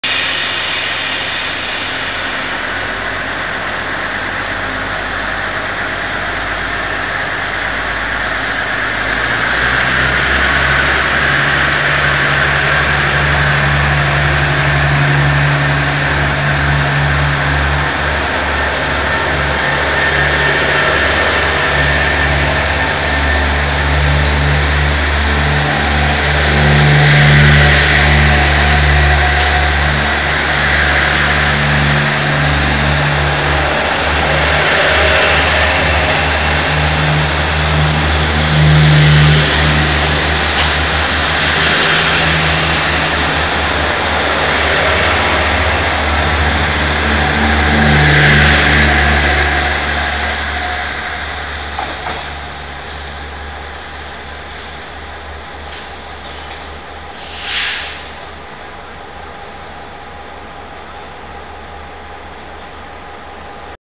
台鐵213次DR3000型自強號離站(花蓮)
台鐵213次DR3000型自強號離站(花蓮),攝於2014年5月23日,去拍火車囉~